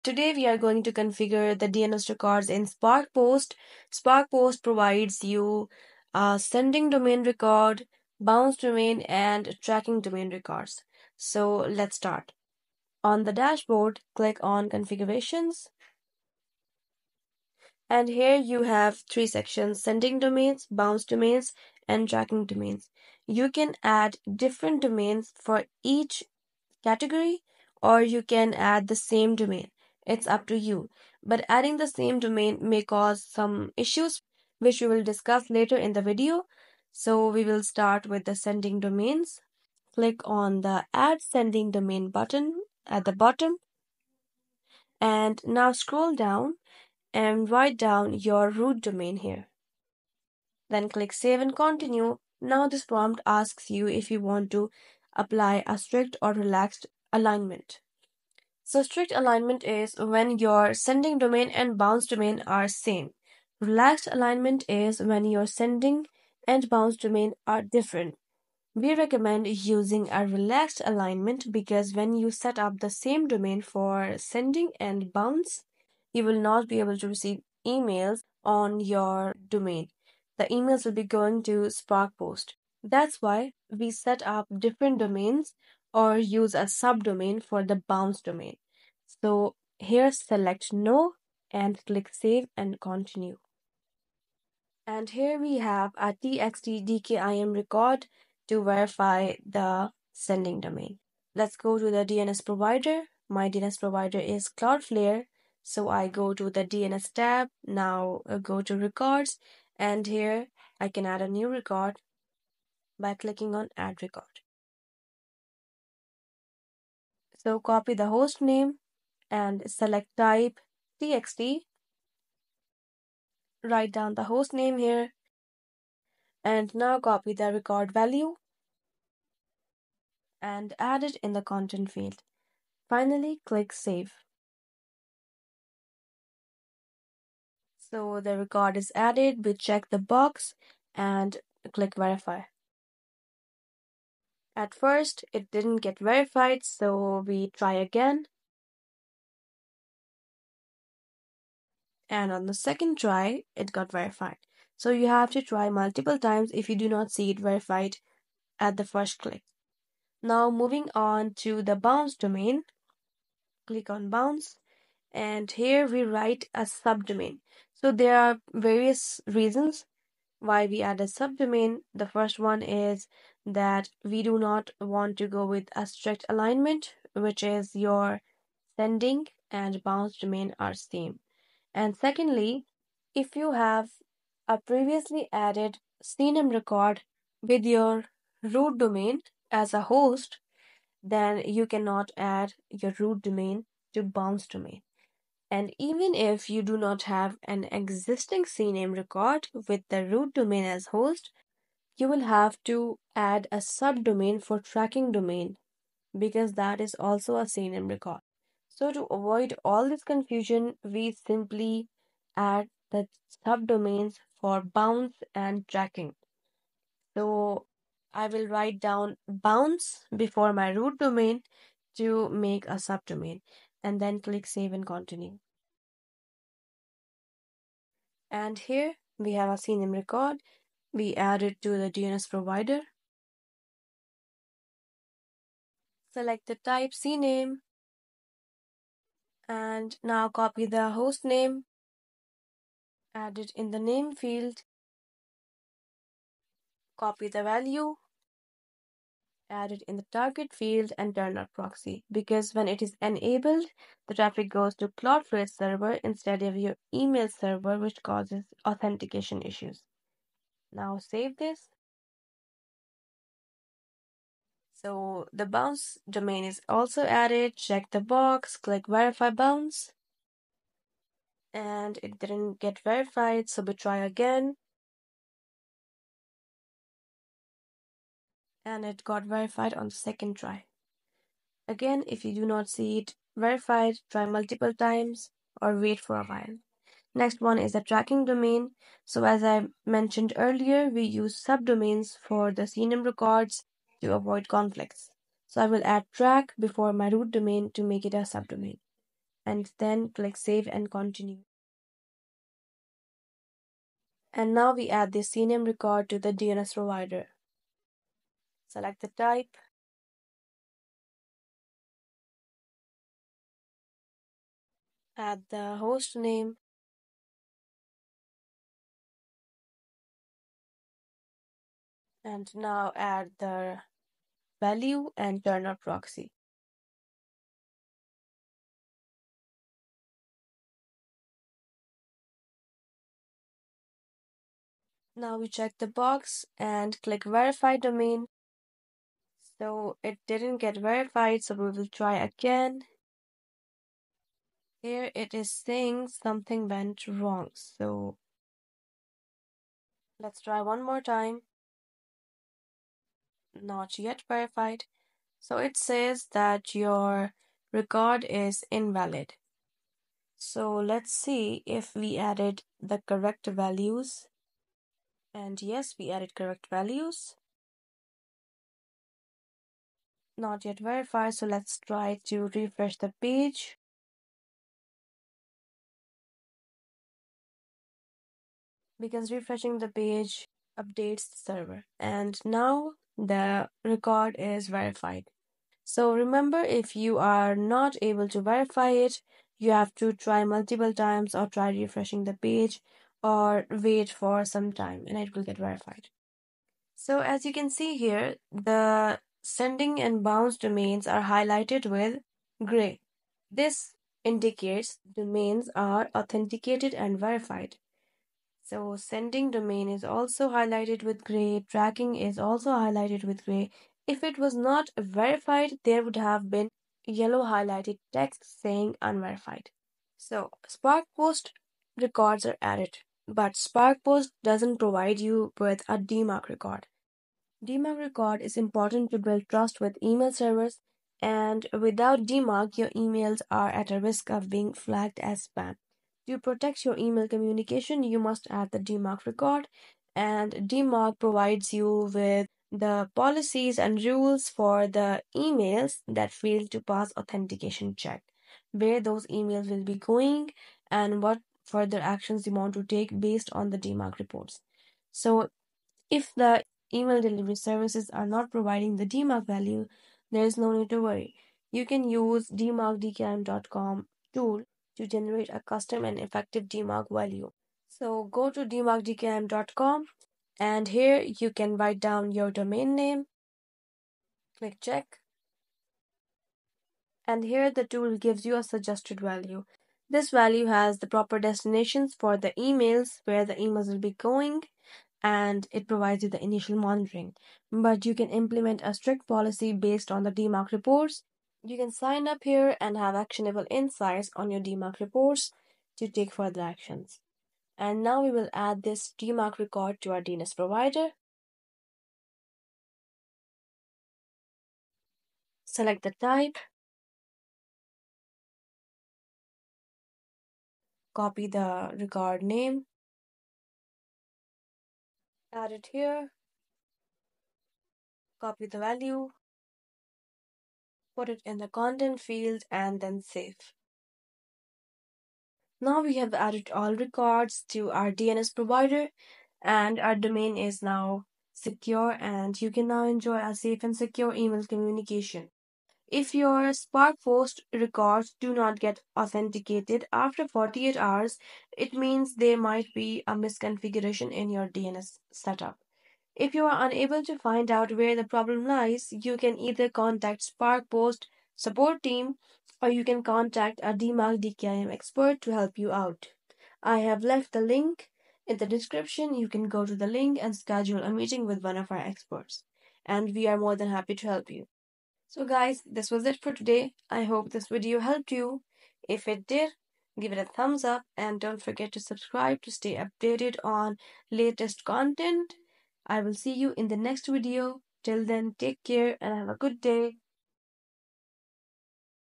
Setup Domain Authentication for Sparkpost - Sending, Bounce, and Tracking Records
Hello there! Follow the steps in this video to authenticate sending domain, bounce domain, and tracking domain.

Add the TXT and CNAME records to the DNS provider to verify DNS records and authenticate your email domain.